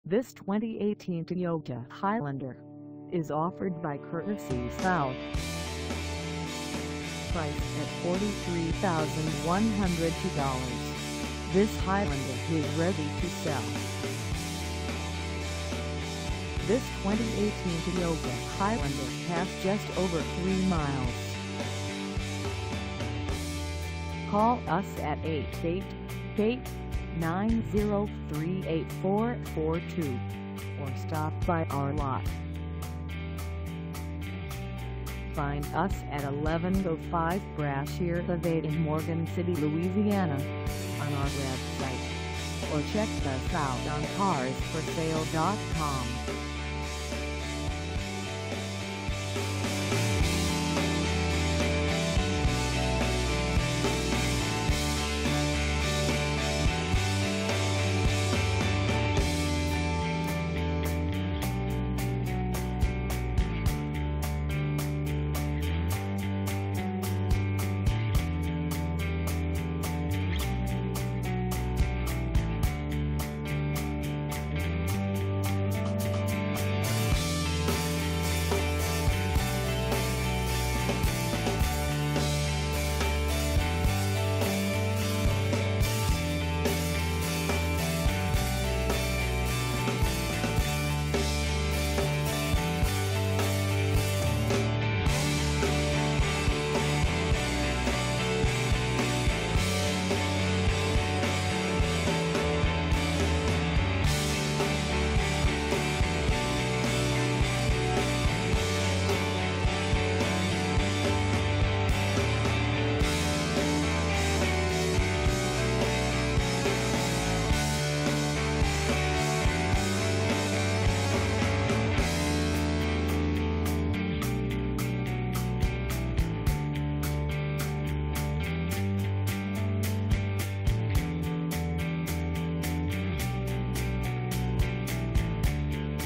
2018 Toyota Highlander XLE 4dr SUV for sale in Morgan City,
This 2018 Toyota Highlander XLE 4dr SUV is for sale in Morgan City, LA 70380 at Courtesy  South.

Engine: 3.5L V6
Transmission: Automatic 8-Speed
Color: Celestial Silver Metallic
Mileage: 3
Contact Courtesy  South 1105 Brashear Ave Morgan City, LA 70380 to test drive this 2018 Toyota Highlander XLE 4dr SUV today.